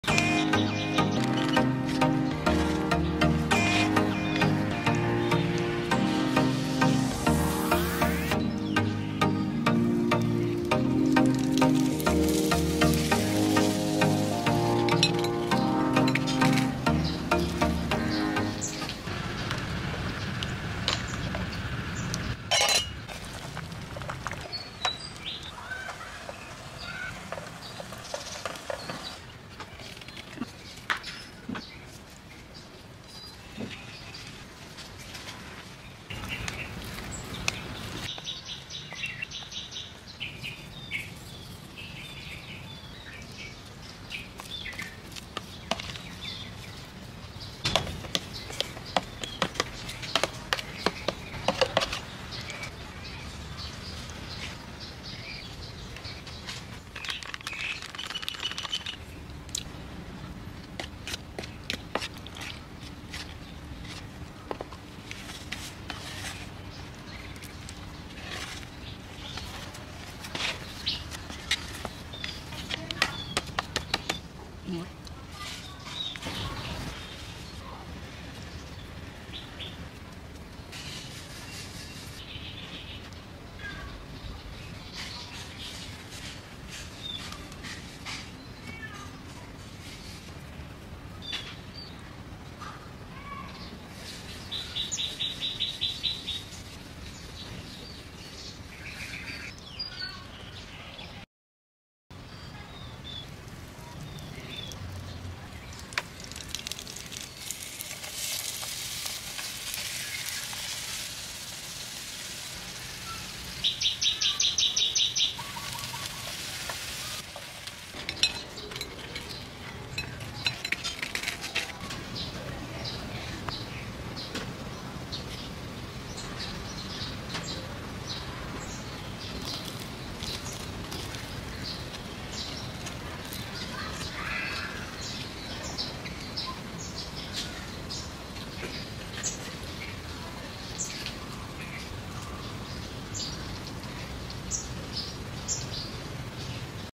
Tasty Glutinous Rice Cake | Crispy Glutinous Flour Filling Sweet Potato Dessert | Easy Recipe
This recipe is really easy and delicious. Try it if you like it too.

Ingredients:
- Glutinous Rice Flour
- Sweet Potato
- Fresh Milk
- Sugar
- Salt